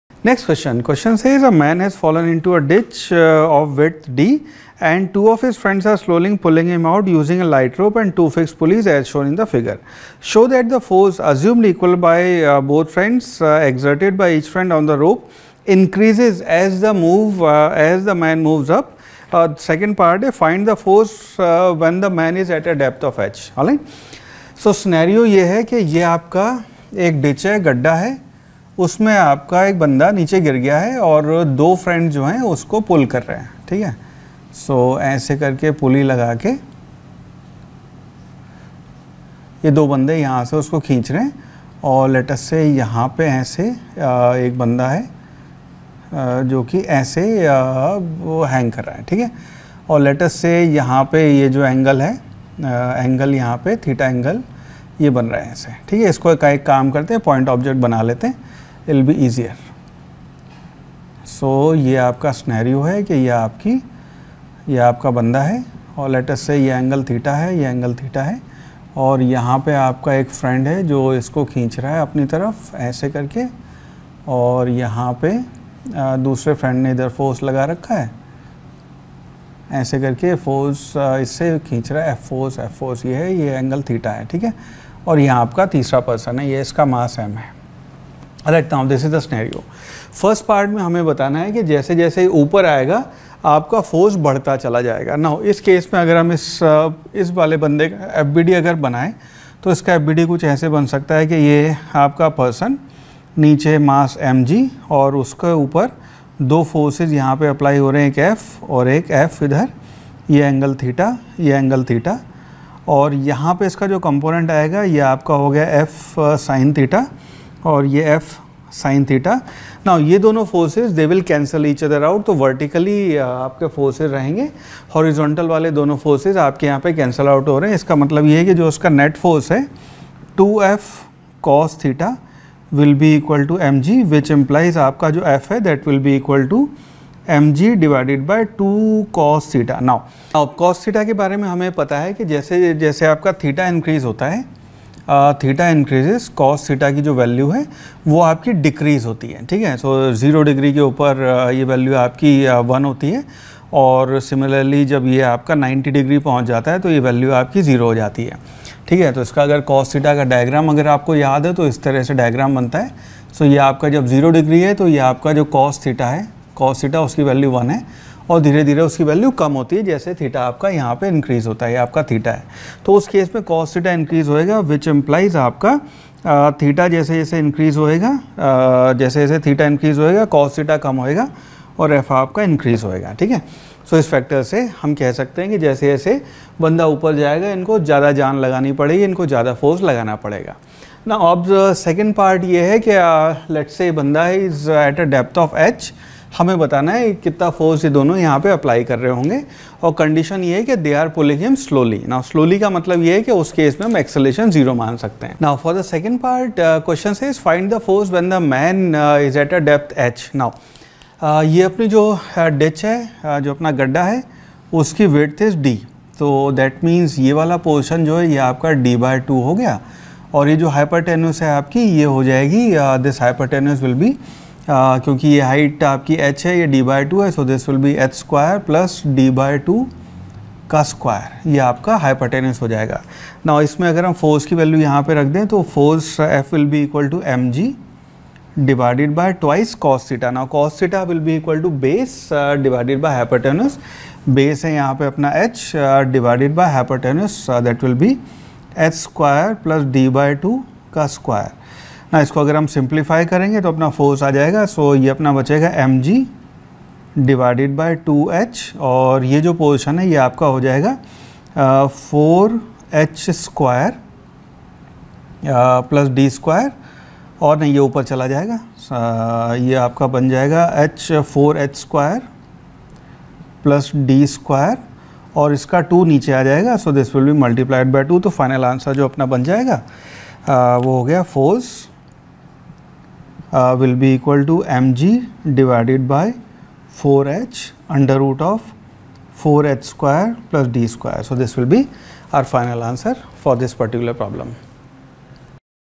HCV: A man has fallen into a ditch of width d and two of his friends are slowly pulling him out usin
A man has fallen into a ditch of width d and two of his friends are slowly pulling him out using a light rope and two fixed pulleys as shown in figure. Show that the force (assumed equal for both the friends) exerted by each friend on the rope increases as the man moves up. Find the force when the man is at a depth h.